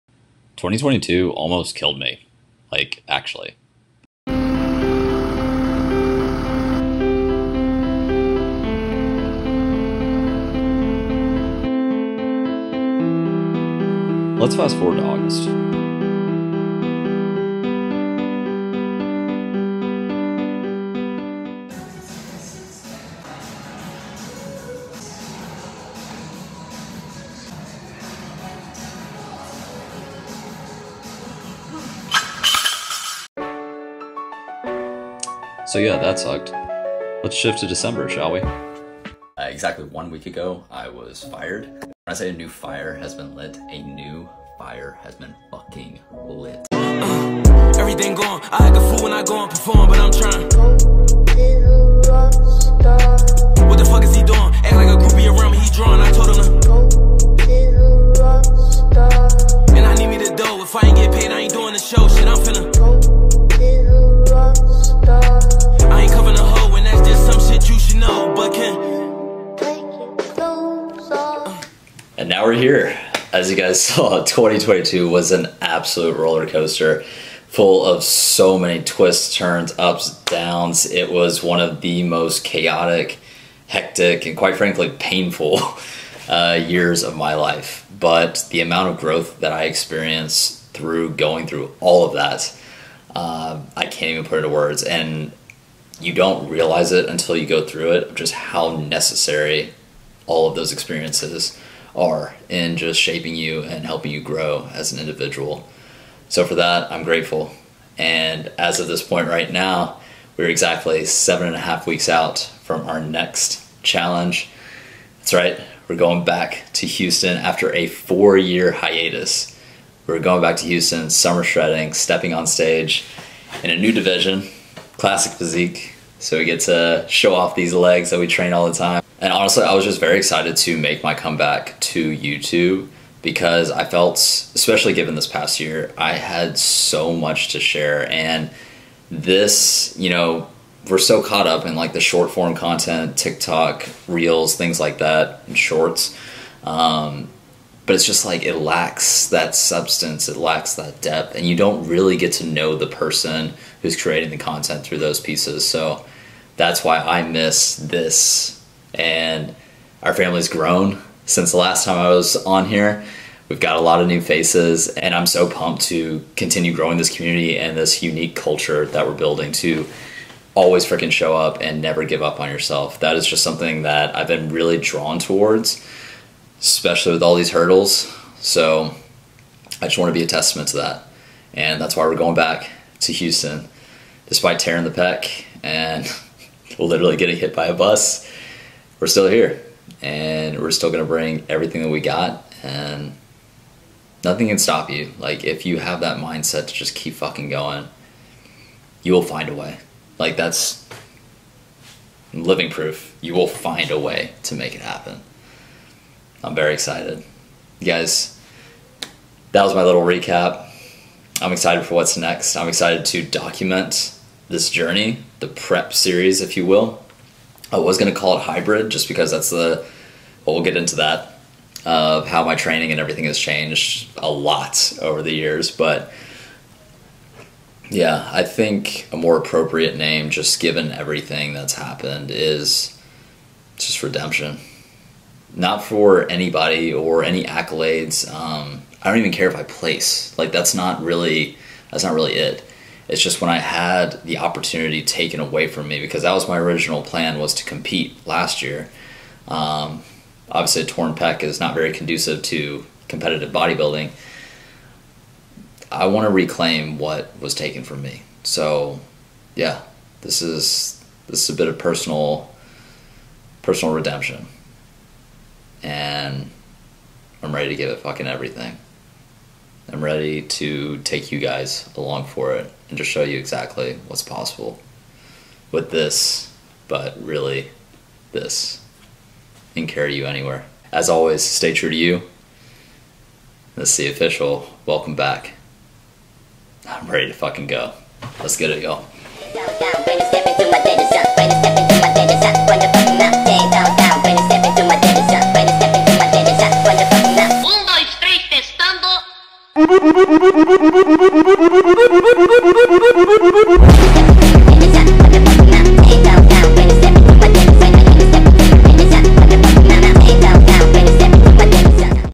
2022 was the hardest year of my life.
This is the official comeback and start of our new series: REDEMPTION

2022 tested me in ways I never thought possible.. This is a very cursory overview of the hurdles I experienced and exactly how I chose to combat them to rise above.

When you give yourself no other option but to overcome the challenges you're faced with - you become unstoppable.

This new series and quite frankly channel as a whole will be a testament to that ideal.

NEVER give up on yourself. I promise you will make it through.

I also create culture for brands. Hit up to learn more and snag some merch if ya think it's dope 🤙🏽